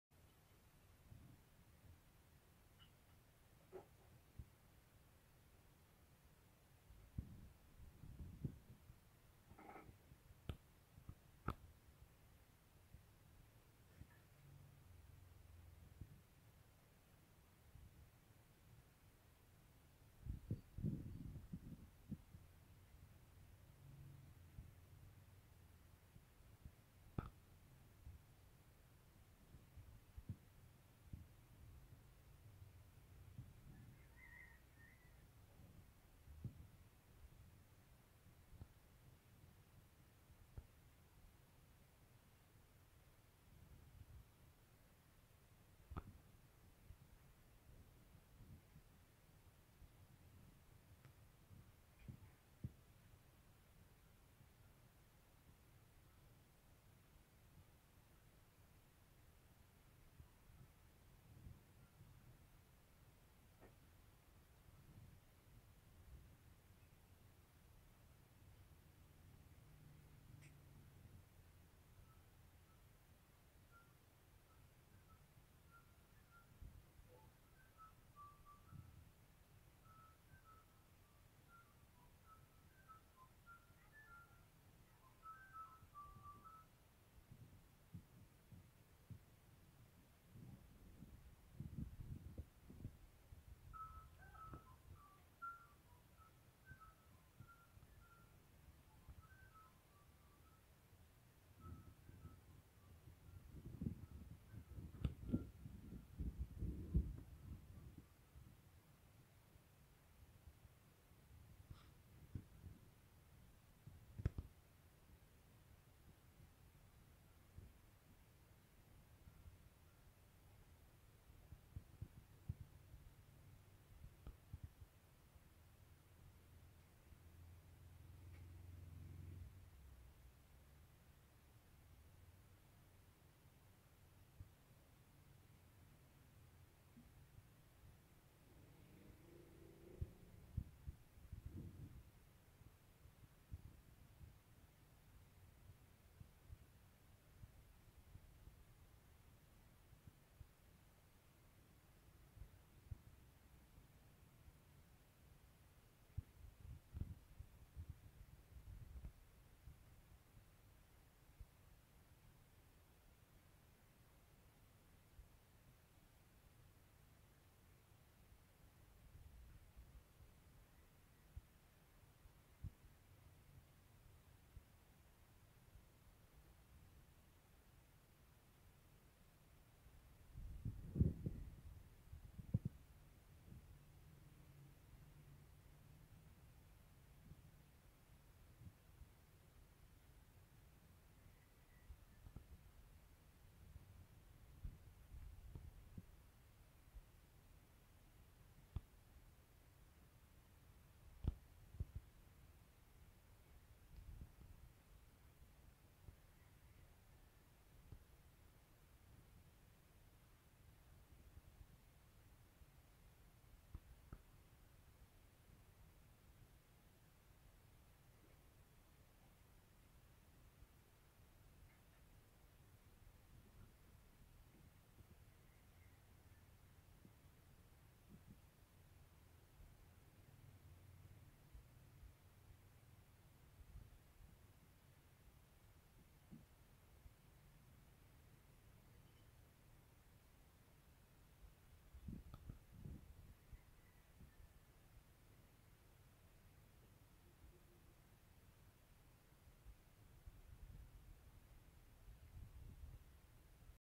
Spider Fight(Winner Takes All)
2 orb weaver spider fighting for there life🖒